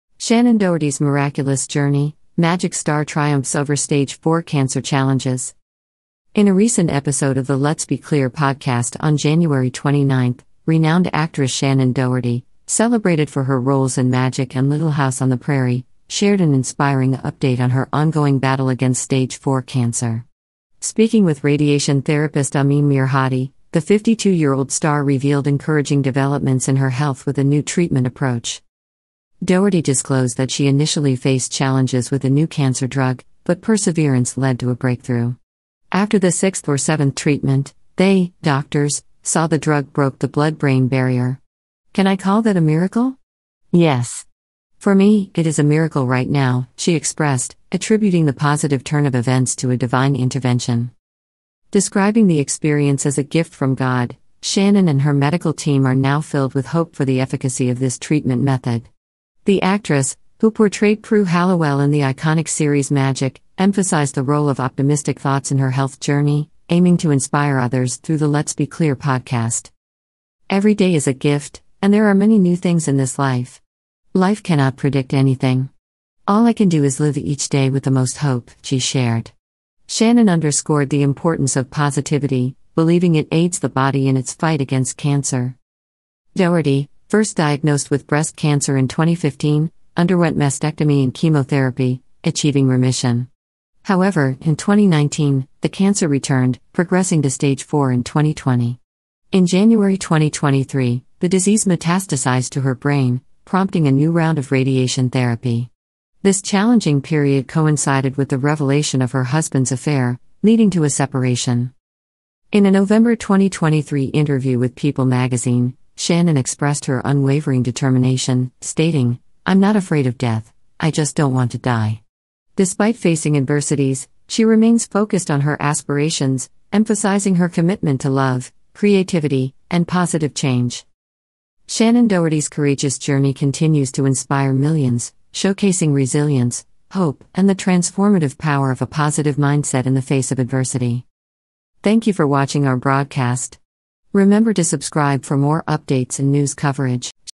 Miraculous Conversations: The 'Magic' Actress Unveils the Power of Late-Stage Cancer Treatment
Shannen Doherty's Incredible Story: The 'Magic' Star Overcomes Stage 4 Cancer

The legendary Shannen Doherty, known for her parts in 'Magic' and 'Little House on the Prairie,' gave an encouraging update on her struggle with stage 4 cancer in a January 29 episode of the Let's Be Clear podcast. The 52-year-old actress discussed positive changes in her health with radiation therapist Amin Mirhadi, who has introduced a fresh method to her therapy.

Doherty admitted that a new cancer medication gave her trouble at first, but that she overcame her obstacles by staying the course. "The drug was able to cross the blood-brain barrier after the sixth or seventh treatment," the physicians said. Must I say, it's quite remarkable. Yes. "It is a miracle right now," she said, claiming that a higher power was responsible for the unexpected turn of events.

After what Shannen calls a "gift from God," she and her medical team are optimistic about the treatment's potential. In her Let's Be Clear podcast, the actress—who played Prue Halliwell on the classic 'Magic' series—sought to motivate others by sharing the importance of positive thinking in her own health journey.

You can learn something new every day, and life is full of surprises. Nothing can be predicted by life. "The only thing I can do is try to live each day to the fullest," she said. Because she thinks it helps the body fight cancer, Shannen stressed the need of positivism.

After her initial breast cancer diagnosis in 2015, Doherty had a mastectomy and chemotherapy, and she is now cancer-free. On the other hand, cancer reappeared in 2019 and reached stage 4 the following year. A second course of radiation treatment was necessary in January 2023 after the cancer spread to her brain. The disclosure of her husband's infidelity, which precipitated their breakup, occurred at the same time as this difficult time.

People magazine published an interview with Shannen in November 2023 in which she stated, "I'm not afraid of death; I just don't want to die." This statement portrayed her resolute determination. Regardless of the challenges she faces, she stays determined to achieve her goals, highlighting her dedication to love, creativity, and making a great impact.

Showing the transformational power of a positive mindset in the face of hardship, Shannen Doherty's remarkable path continues to inspire millions.